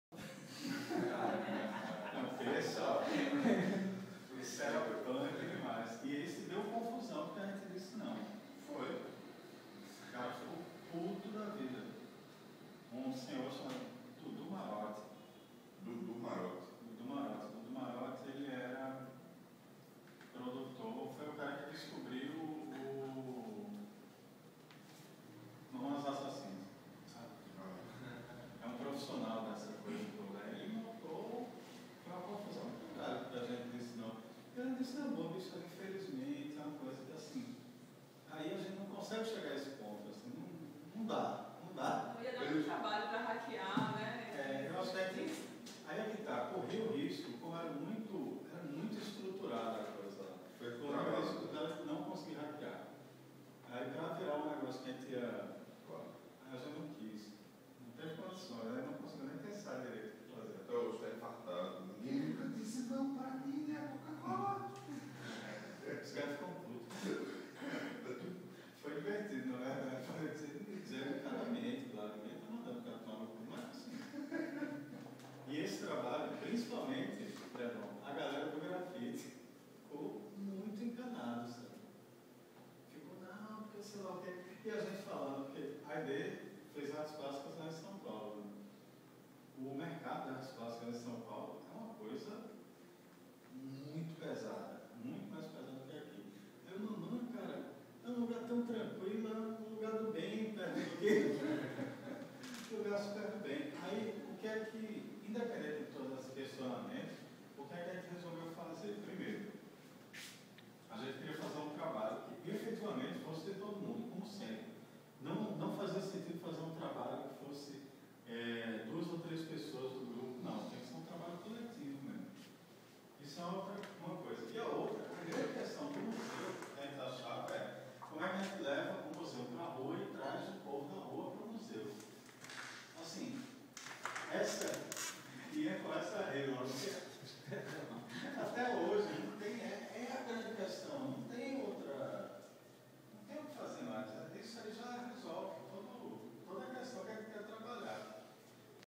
Re:Re:Combo I
Quinta-feira, dia 22 de dezembro 2011, 19horas, H.D. Mabuse, Haidee Lima,  membros remanescentes do coletivo Re:Combo, pela primeira vez desde o encerramento das suas atividades, estarão em Bê Cúbico, para um bate-papo com a exibição de farta documentação dos trabalhos do coletivo.O Re:Combo foi uma experiência de produção de audiovisual coletiva e colaborativa que durou de 2002 até 2008. Começando sua atividade no Recife, como um grupo de produção musical, estendeu suas ações rapidamente para campos tão distintos quanto instalações, web art e remix de licenças de uso legais.

Grupo de colaboração criativa, idealizado em Recife, o coletivo Re:Combo chegou a contar com cerca de 60 integrantes. Gente espalhada pelos 4 cantos do Brasil e outros países. A produção era discutida e organizada numa lista de discussão e via e-mail, ideias, trechos de músicas, imagens e pedaços de vídeos, circularam entre o segundo semestre de 2001 e fevereiro de 2008.